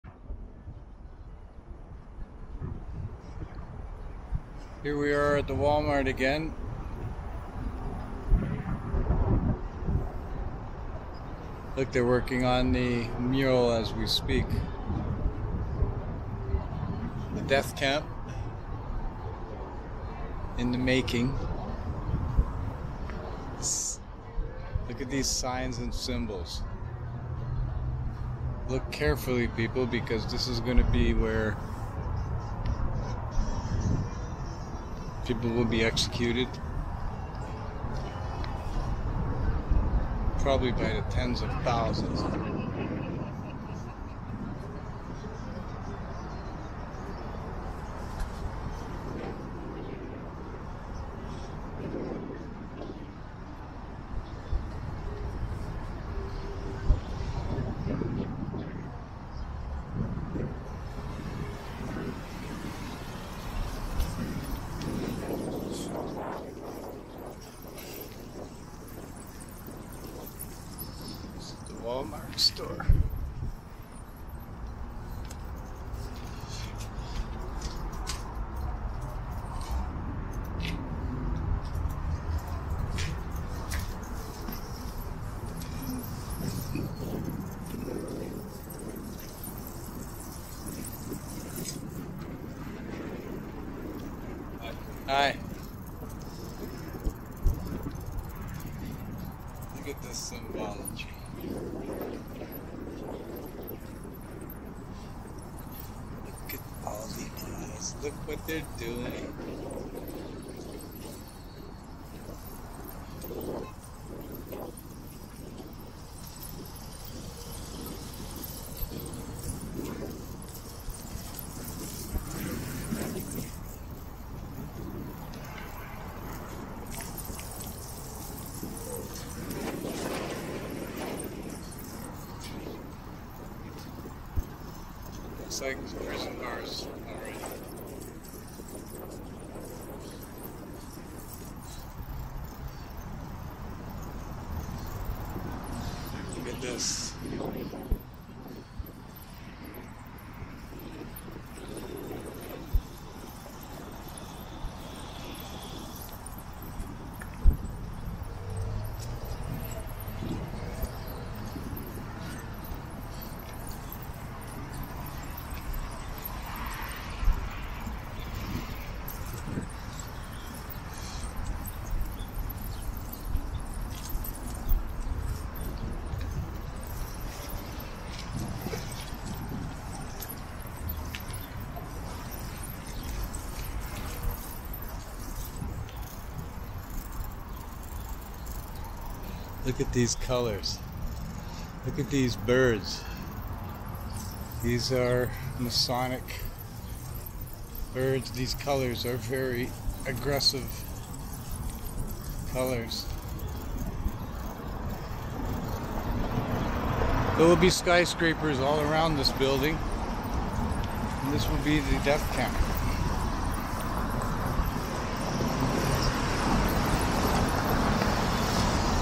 Masonic Walmart latest murals
In plain sight, Masonic symbolism being painted as murals on walls on a shut-down Walmart, obvious to anyone who pays attention to Masonic signs and symbols, this is a death camp in the making.